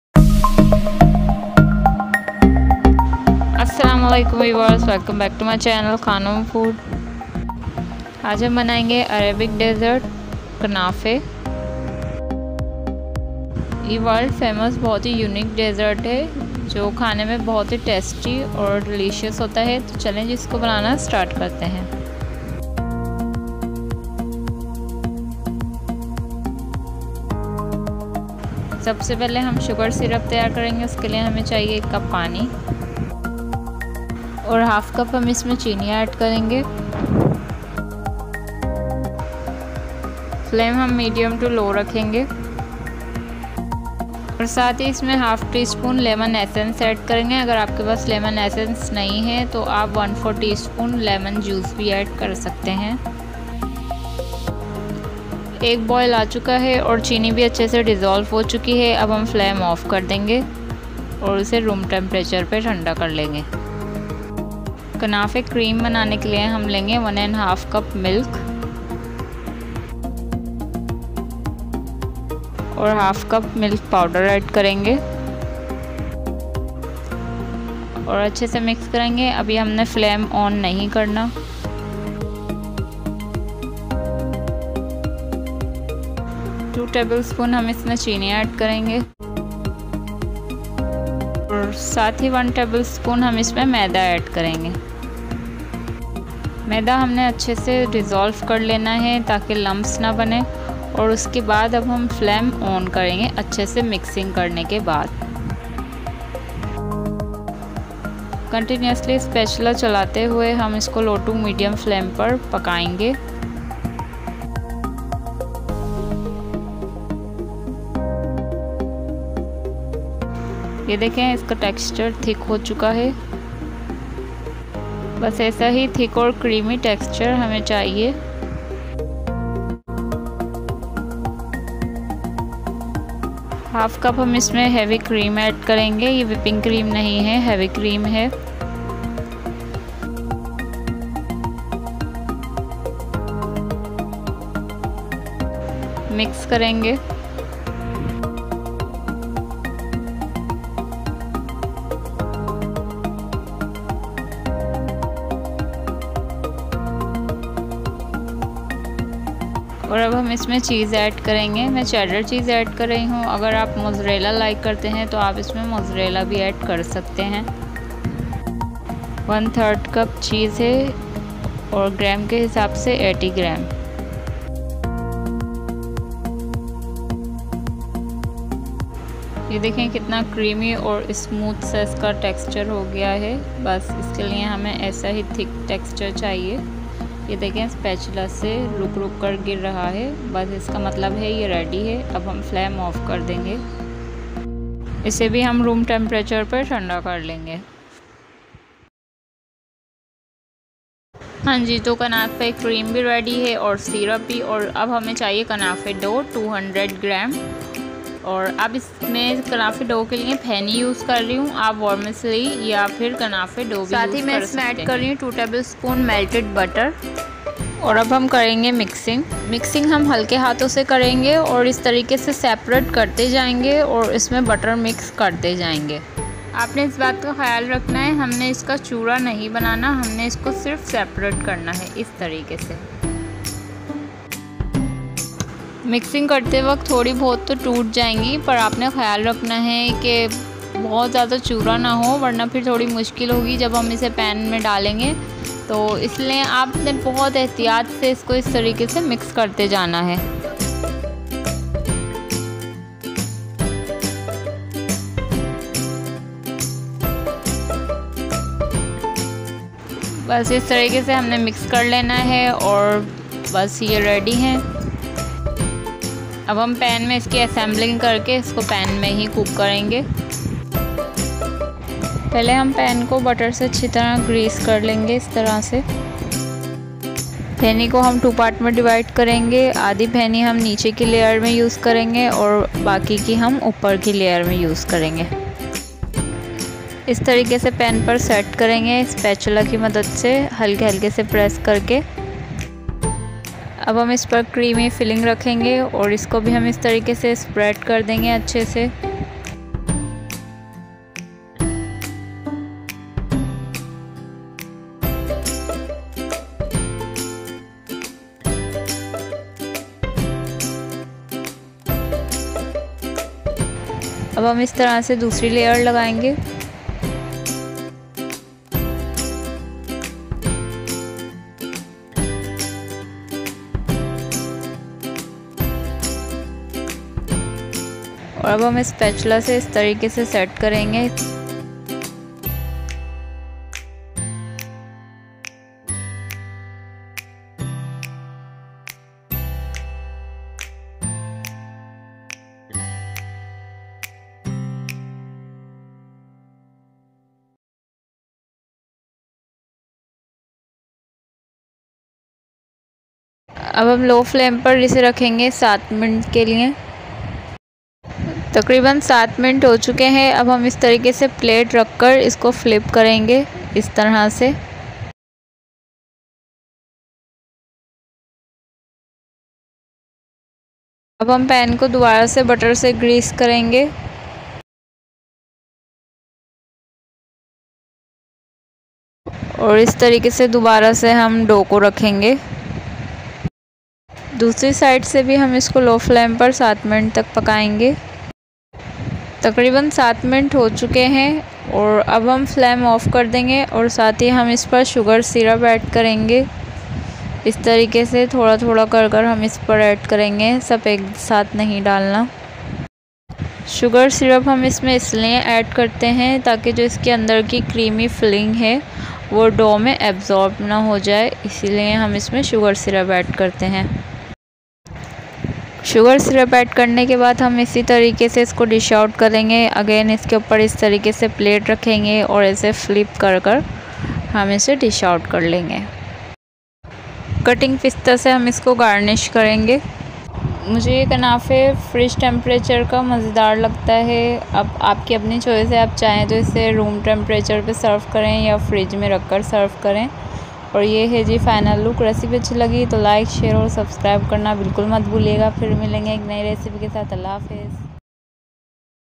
Knafeh Dessert Without Oven || kunafa || Urdu/hindi [Eng subs]
This is world famous dessert kunafeh dessert easy to make recipe without Oven